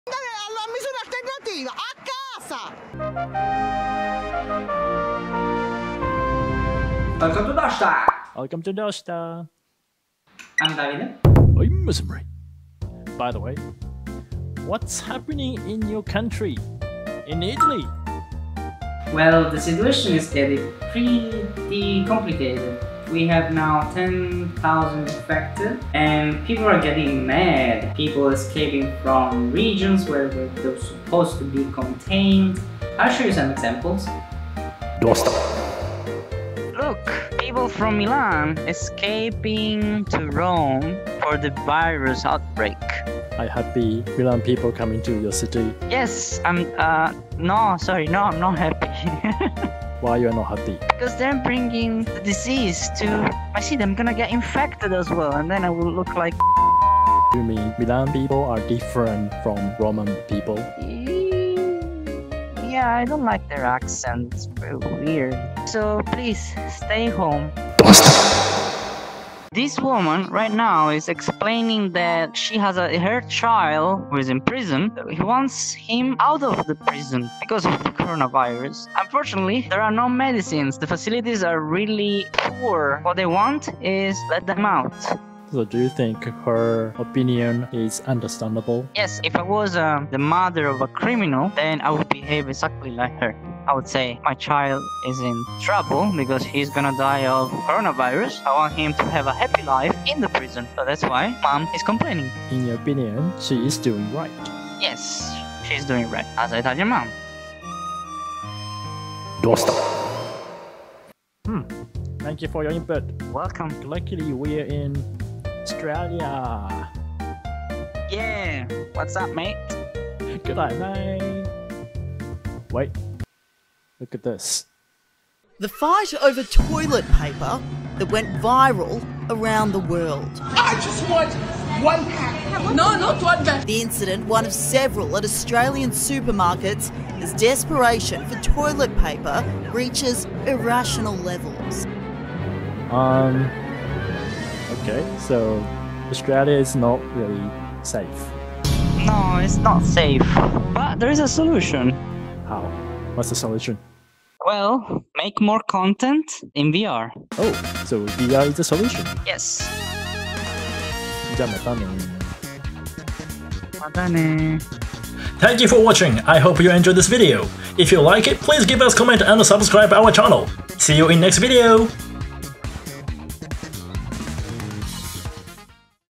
#062 What's happening in Italy!?
Romajin tells his perspective of Coronavirus panic from his local Italian news.